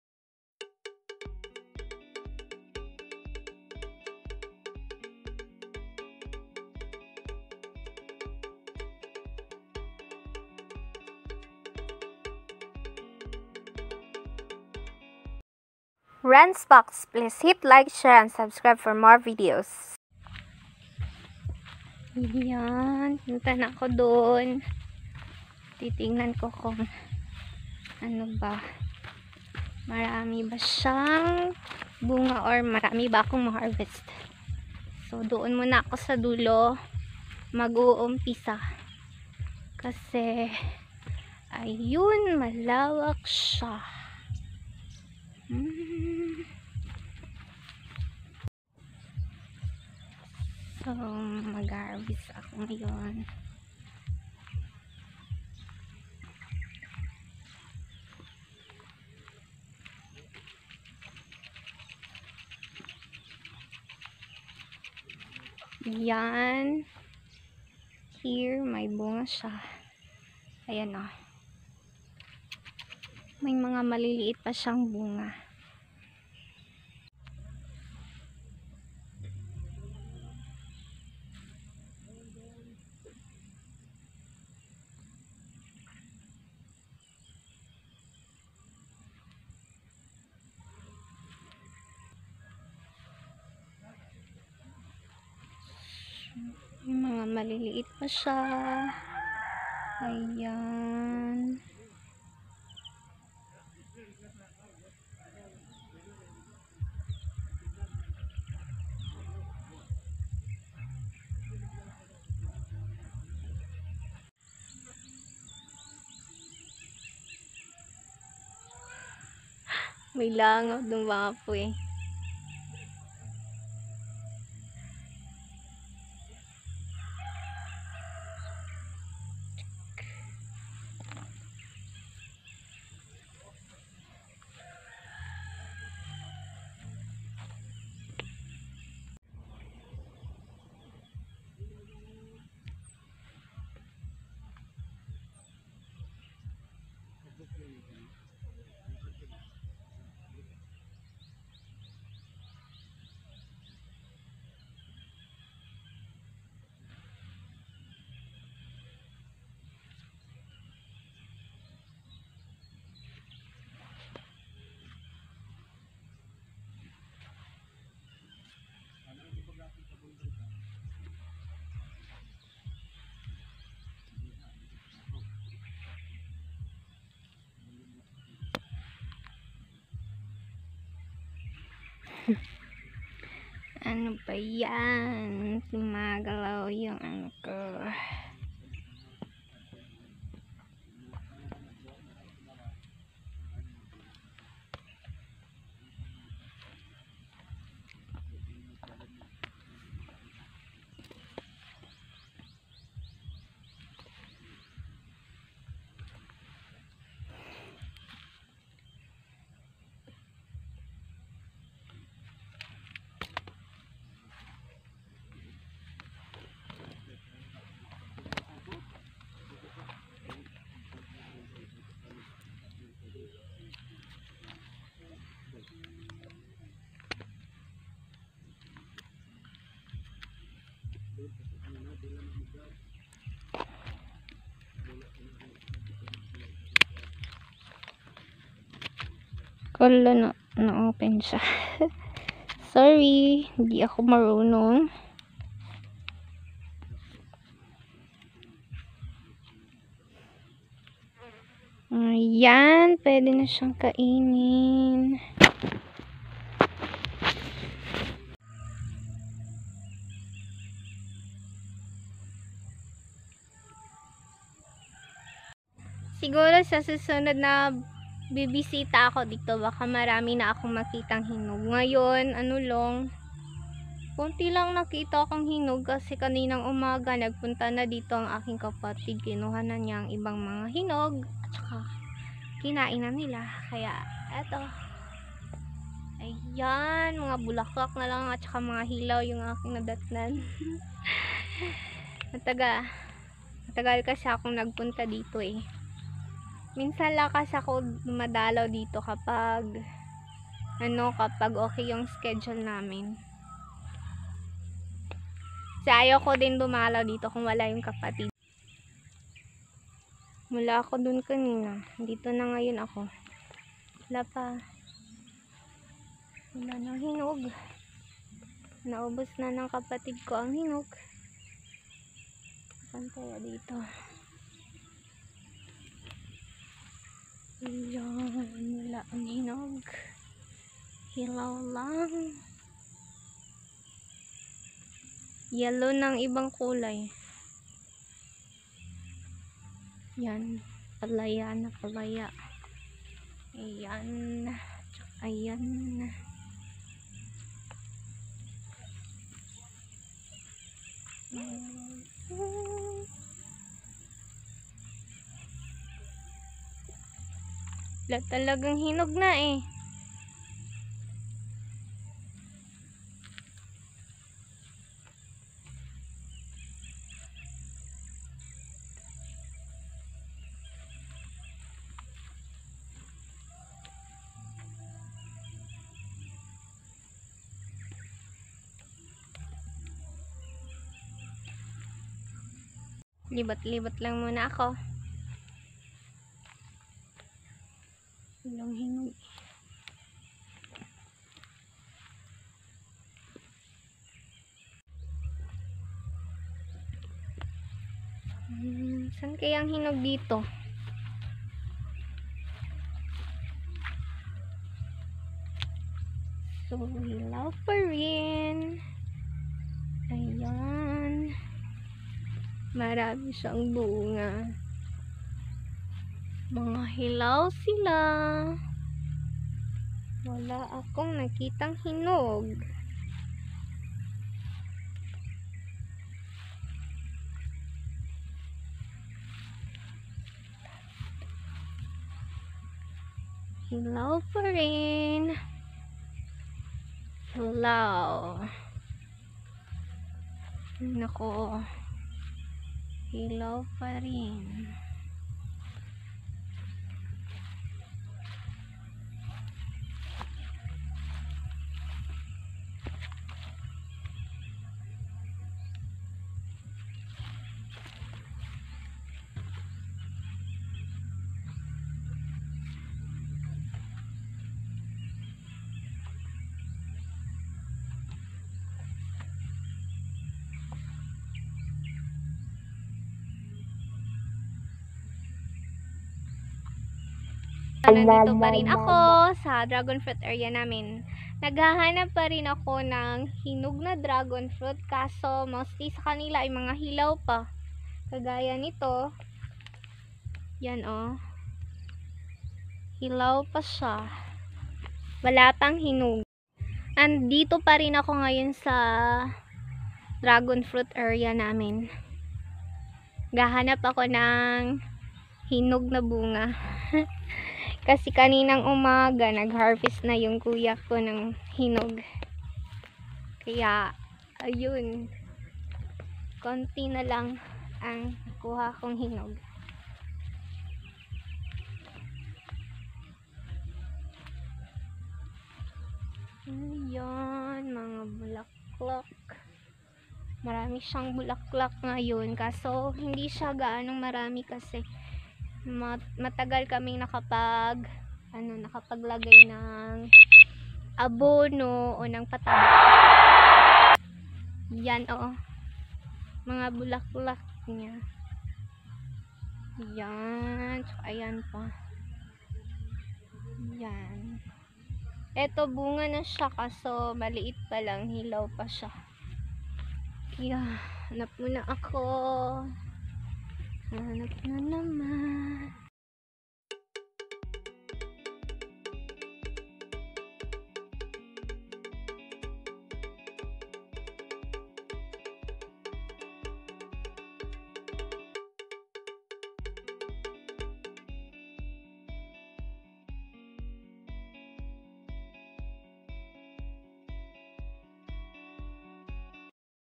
Life in the farm, searching for ripe dragon fruit - (#30)
We all want to find relaxation and experience simple life away from the busy and noisy street. Going to the farm and look for fruits to be eaten.